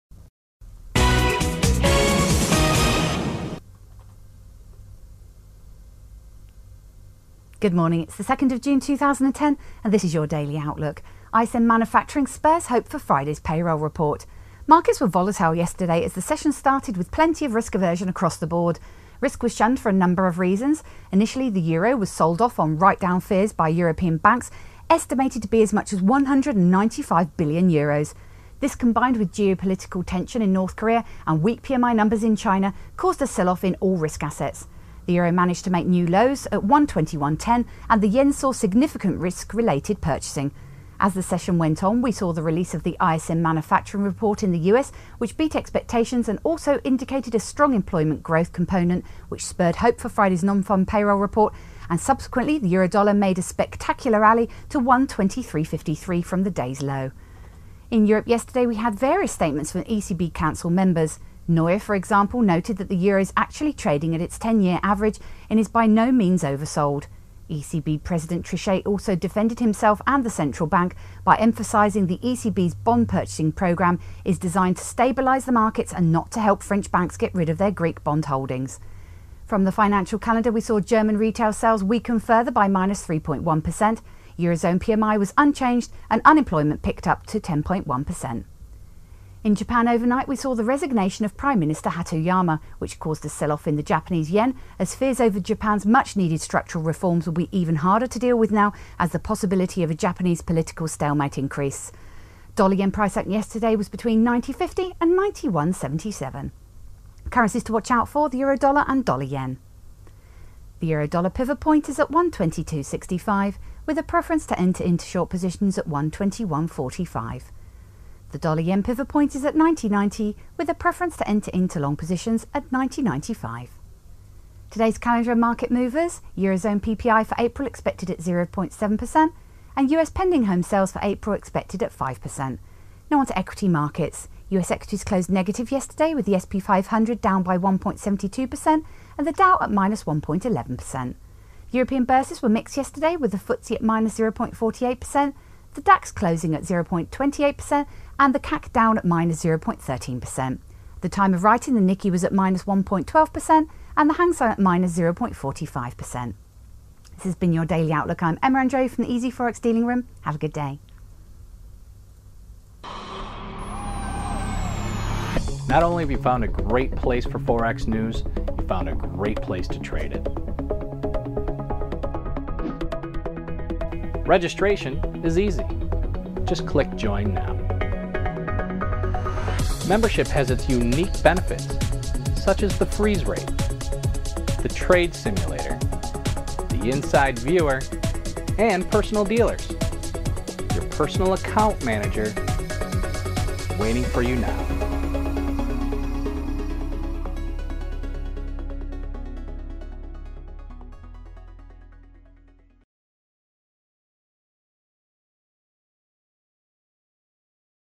easy-forex daily outlook June 2nd, 2010
Easy Forex - ISM Manufacturing spurs hope for Fridays NFP!